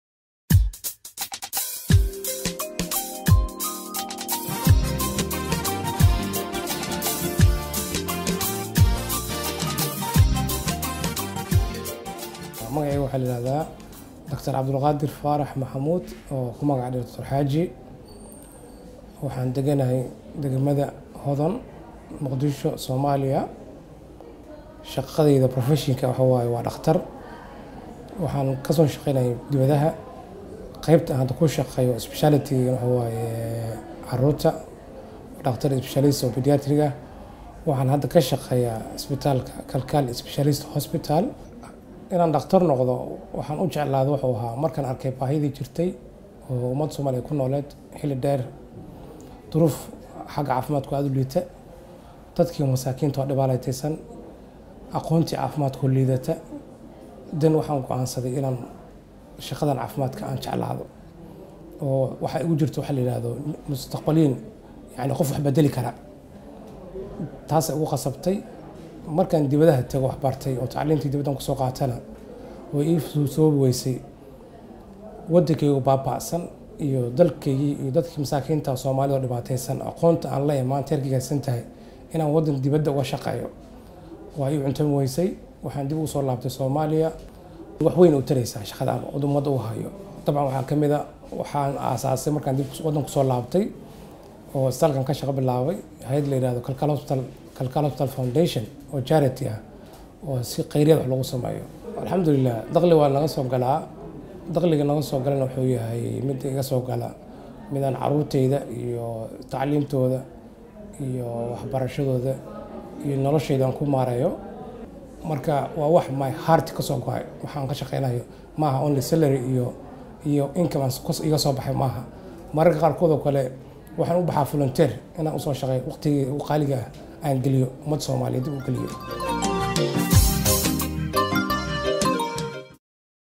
Somali Voices - The Doctor
Challenges in Somalia’s health sector and the overwhelming medical needs of the people inspired Abdulkadir Farah Mohamud’s passion to become a doctor and later convinced him to come home and help rebuild his country.